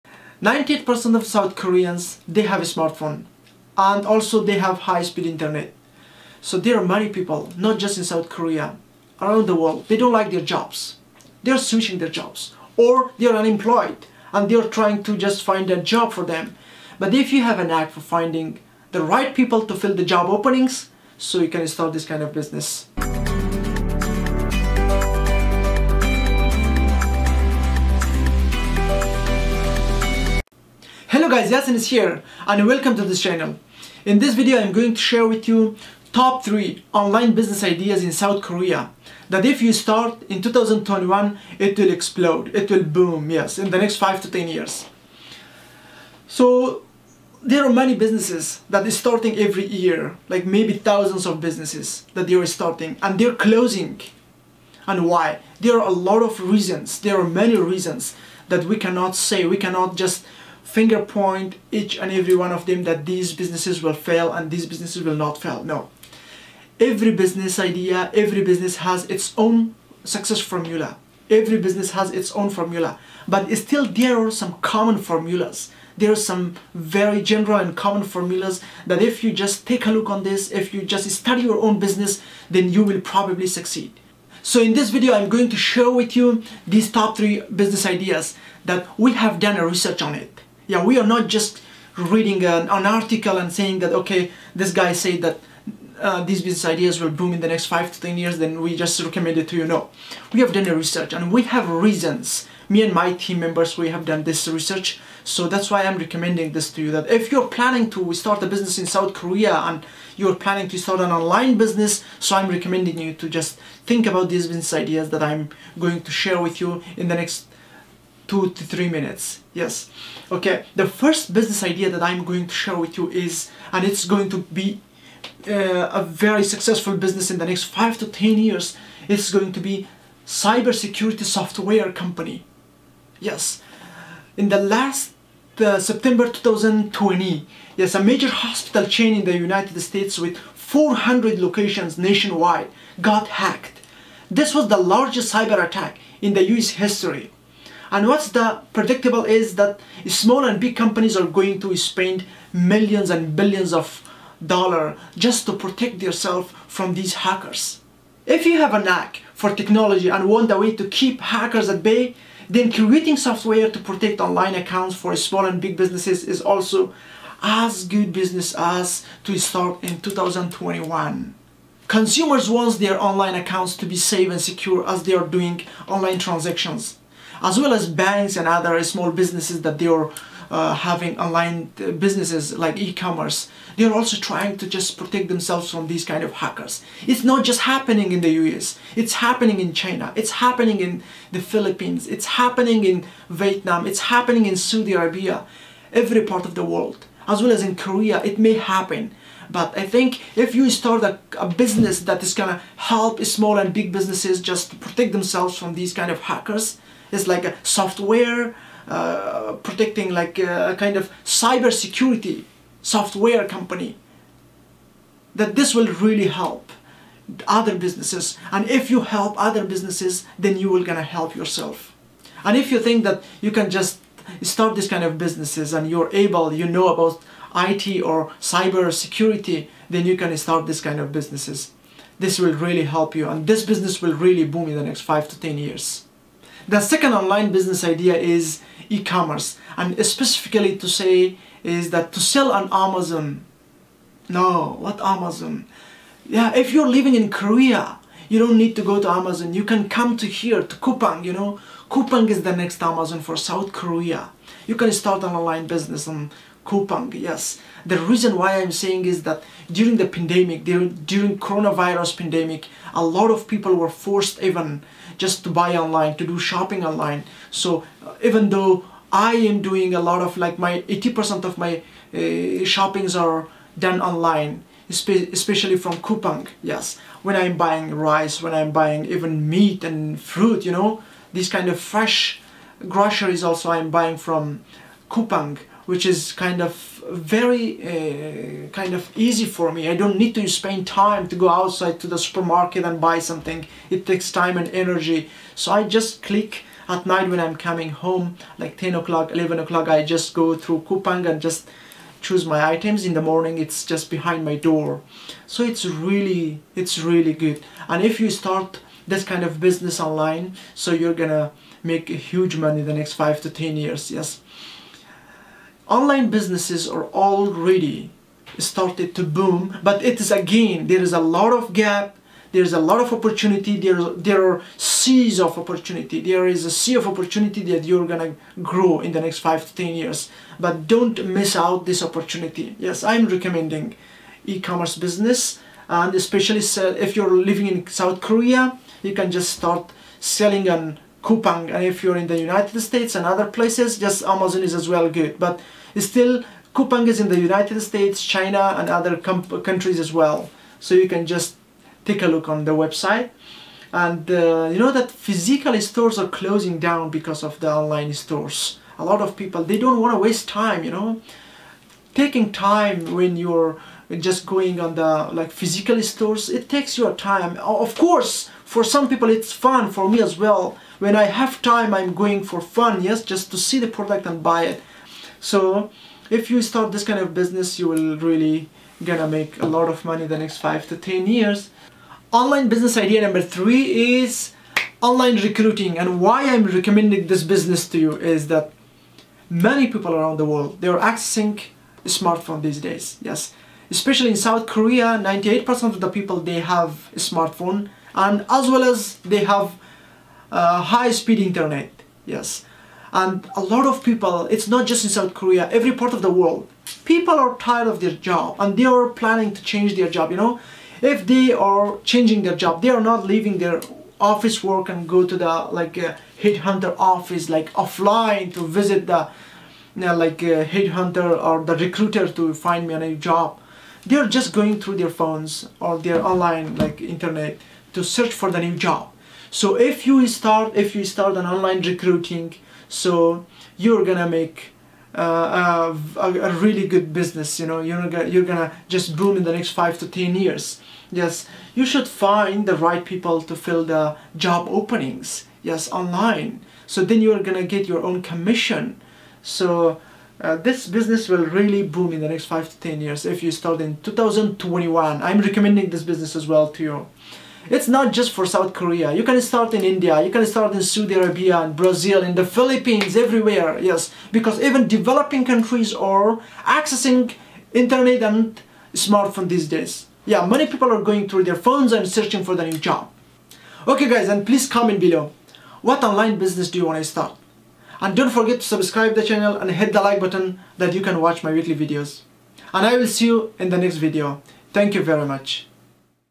Top 3 Online Business Ideas in South Korea in 2021
Recently online Business is booming around the World and if you are in South Korea, these 3 online Business ideas will help you to start your own business in 2021.

You can watch the following videos to learn more about Money, Business and Personal Growth.